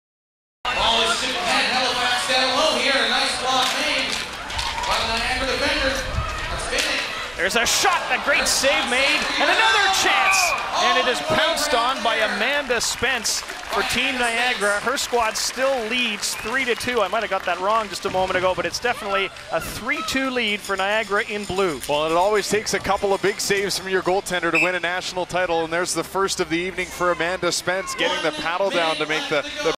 Hockey 2.0 - Amanda Spence One Armed Bandit Save - Niagara Fighters
Play On! 4on4 Redwood Cup National Championship!
Niagara Fighters vs Halifax Highlanders